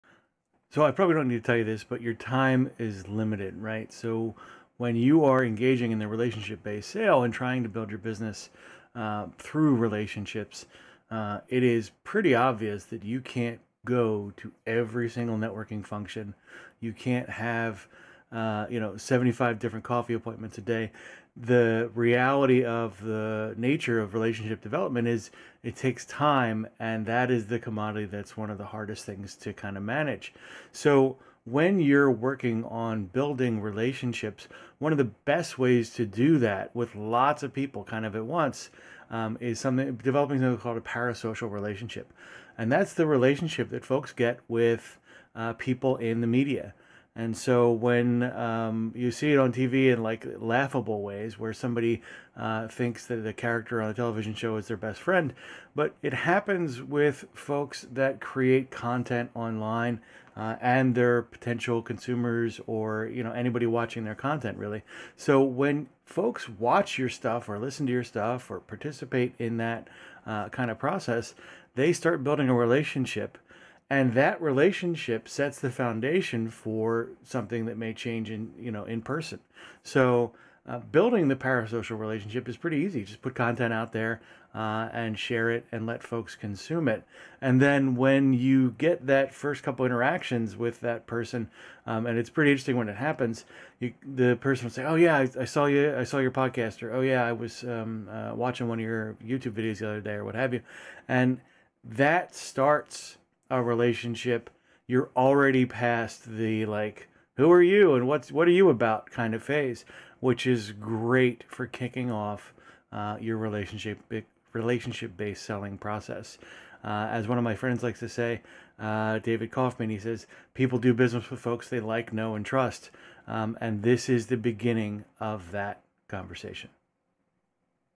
Relationship Based Selling at Volume with Parasocial Relationships
Relationship based selling is awesome!

It's my favorite way to sell services. When you start with a caring relationship, where you want to help other people find the solution to their challenges, you begin the foundation for all sorts of positive outcomes in the future.

The only real drawback with building relationships is time. Building meaningful relationships is time intensive. . . unless. . .  you have some sort of method to asynchronously start a relationship.

Enter podcasts - starting relationships has never been easier now that everyone broadcast content. This type of relationship, called a parasocial relationship is a great way to build trust and rapport with prospects you haven't met yet. It's a deep well of a topic and I'll be posting more on what it is, how it works, and some best practices.